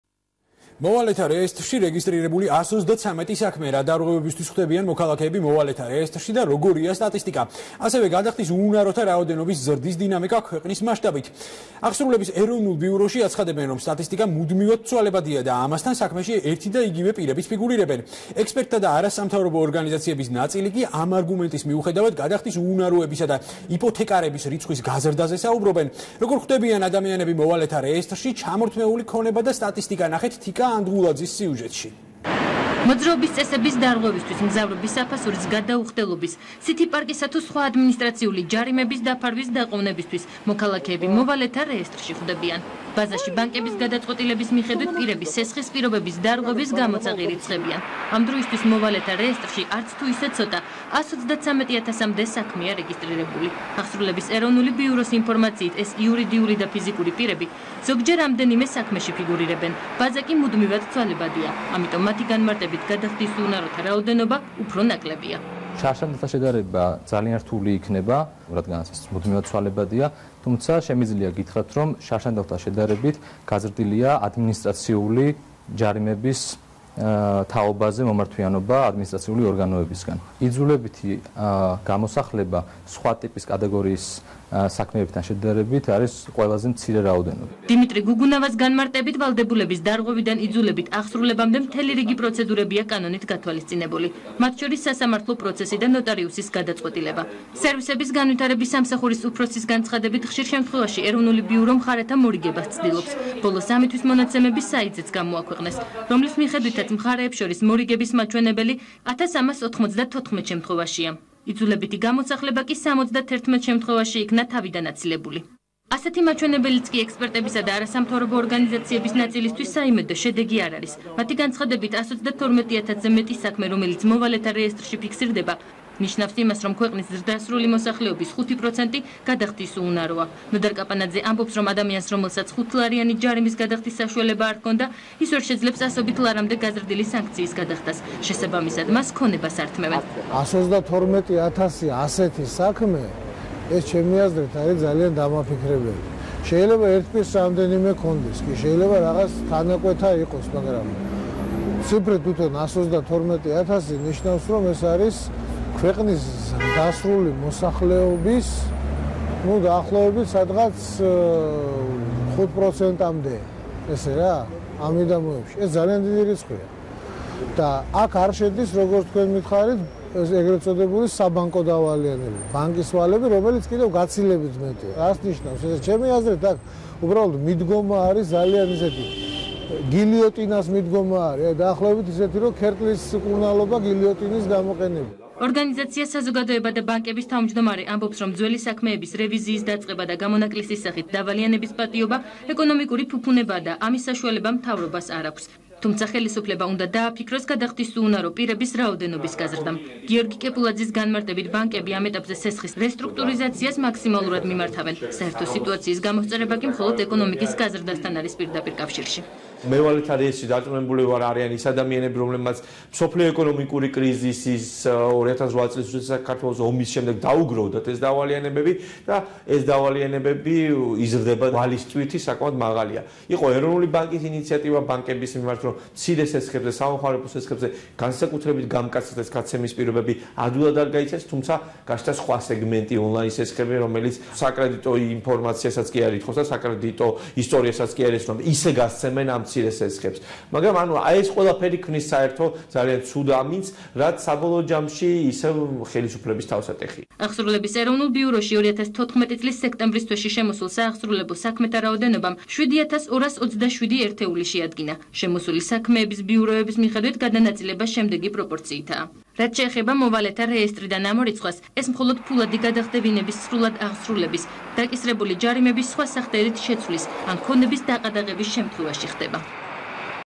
მოვალეთა რეესტრში რეგისტრირებული 133 საქმე, მაესტრო, ბიზნესკონტაქტი
რა დარღვევებისთვის ხვდებიან მოქალაქეები მოვალეთა რეესტრში და როგორია სტატისტიკა?